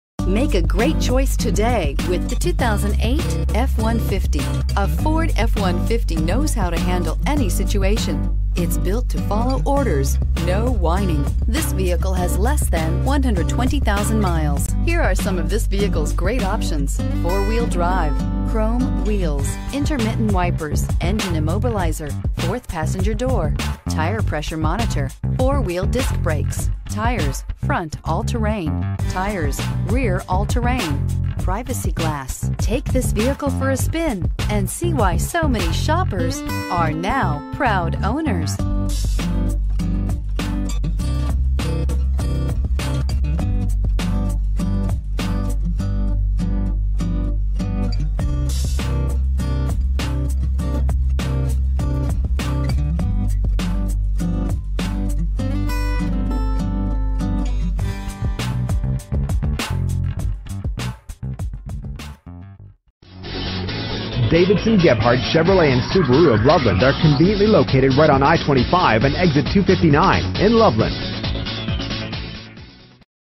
2008 Ford F-150 Loveland CO T19332B
Pick up this Pueblo Gold Metallic 2008 Ford F-150, available today at Davidson Gebhardt Chevy. This could be the one you've been searching for.